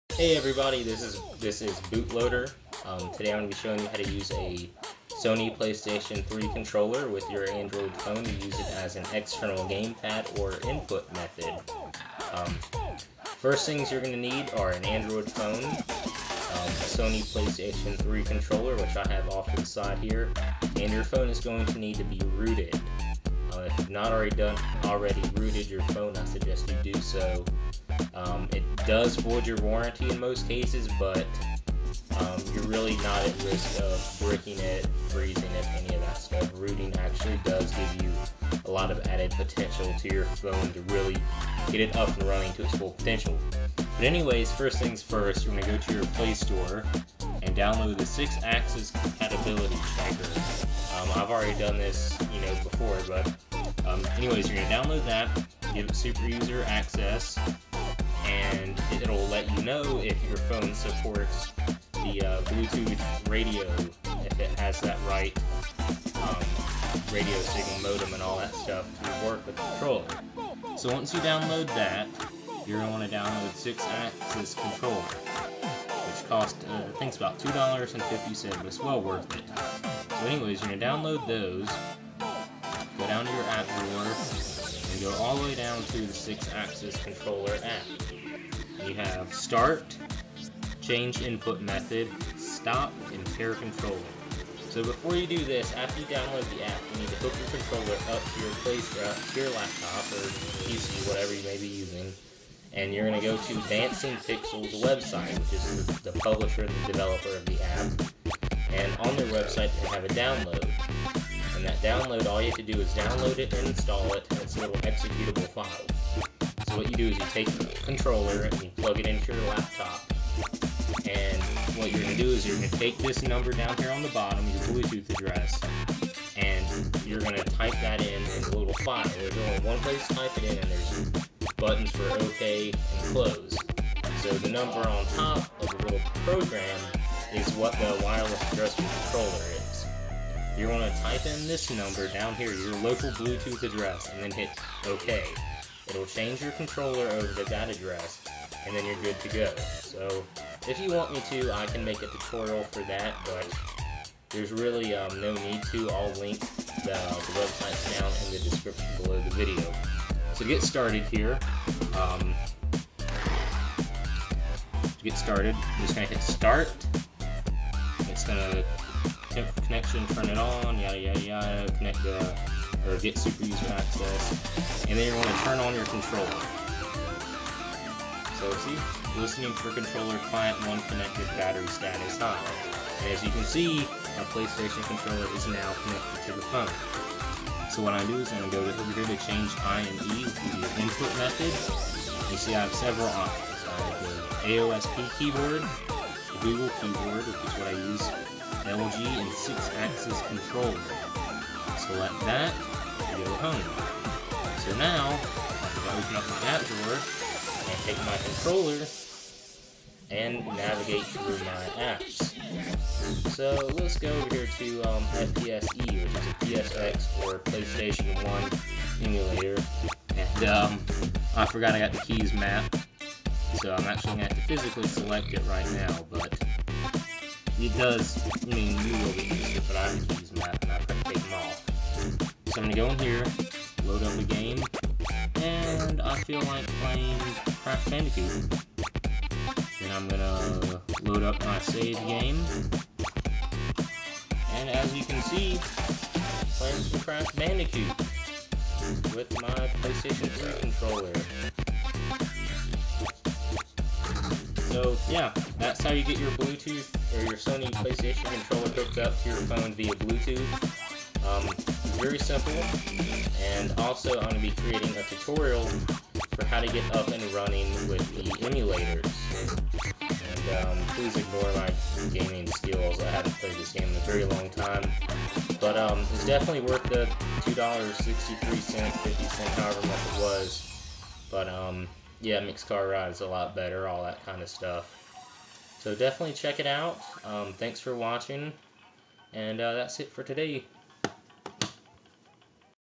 How to connect your PS3 controller to your Android phone.
Make sure to check your phone's compatibility before buying the app.